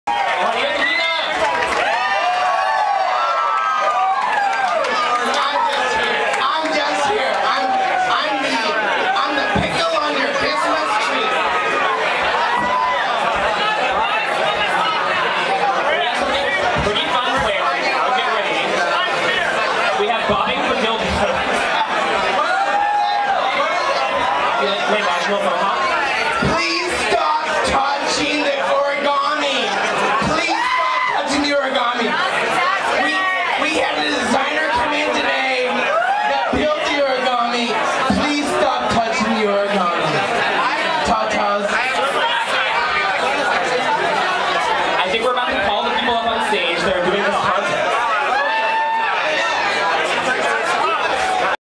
Wet And Wild @ Club 8: Introducing "Bobbing For Dildos" Contest
Lady TaTas and Juanita Fajita dancing in the dark at Club 8 for the WET AND WILD party was off the hook! These two ladies brought it down Saturday night. Introducing the bobbing for dildos contest!

WET AND WILD is every 3rd Saturday @ Club 8 - San Francisco. Check it out @ eightsf.com